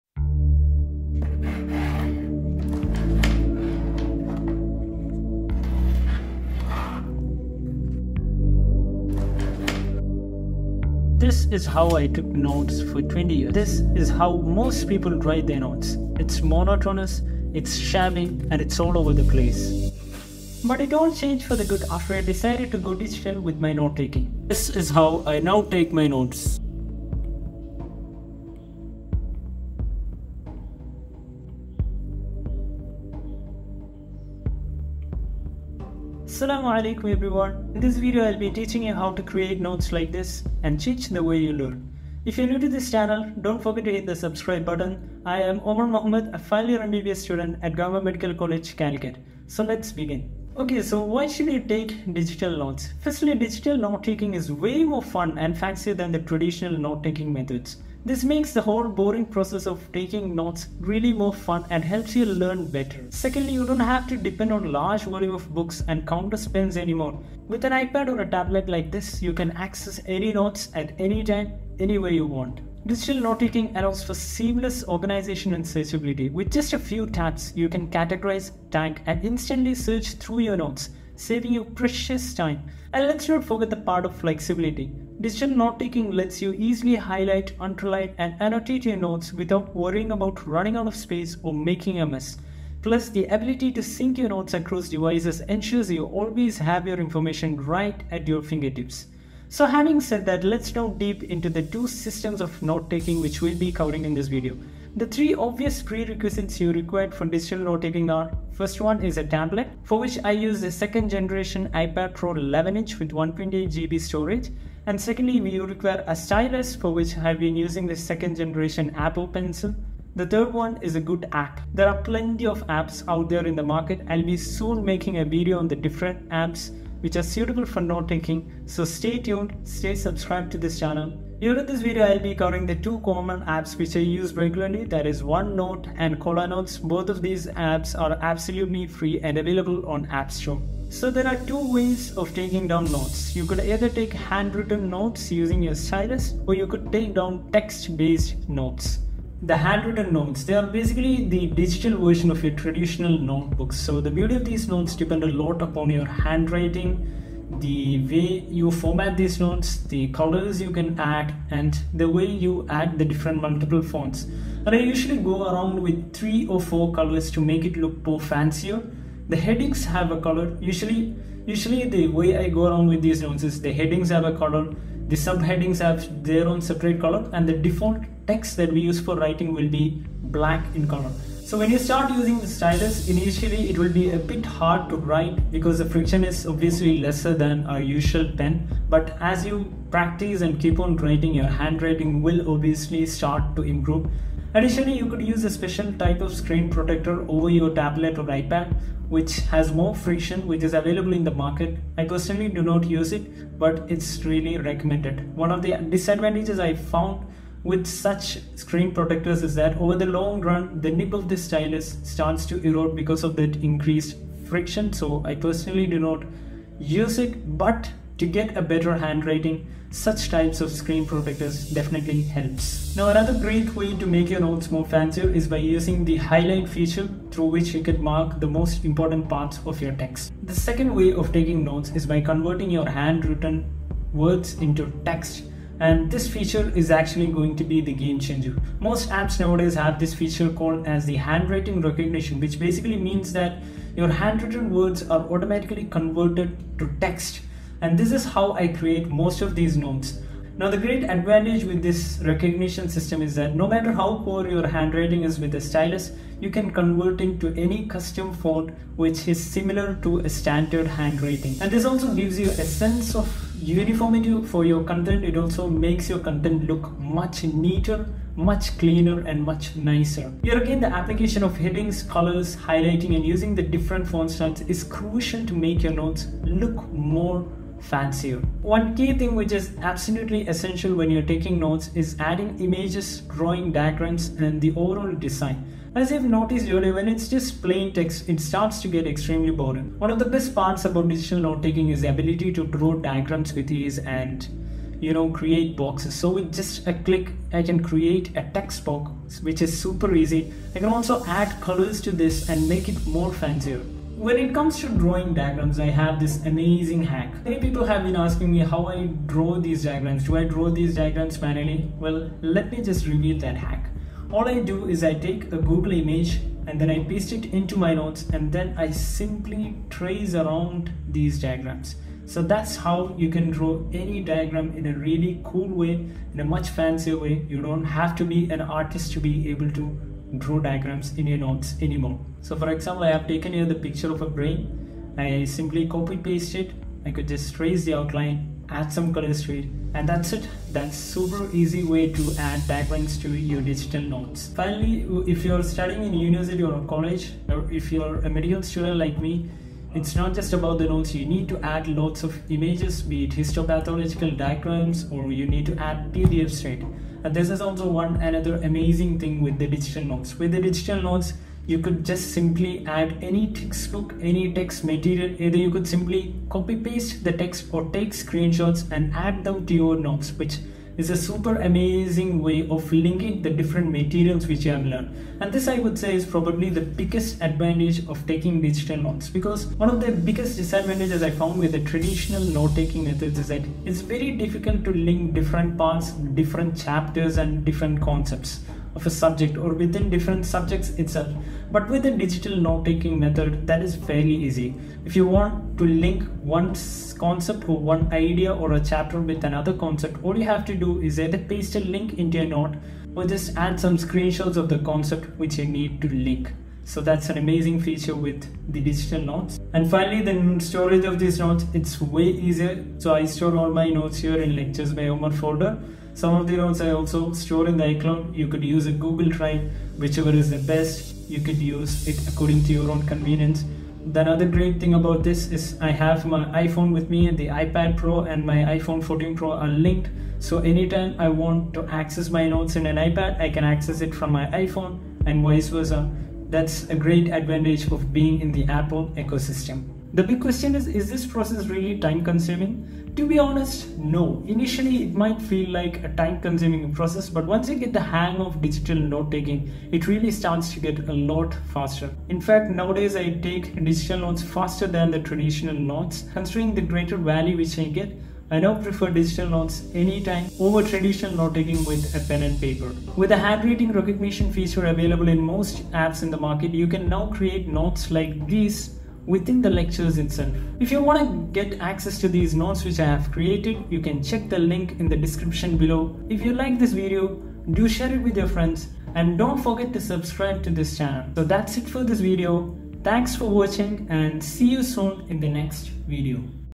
How I Take Notes on my iPad Pro that 5x My Learning
Time stamps for video:

00:00 Introduction
00:52 Why You Should Go Digital
01:46 What You Need To Buy
02:30 Handwritten Digital Notes
03:53 Handwriting-To-Text Feature
04:40 Adding diagrams & designs
05:45 Adding images & PDFs
06:53 Storage & Backup
07:22 Is digital notes worth it?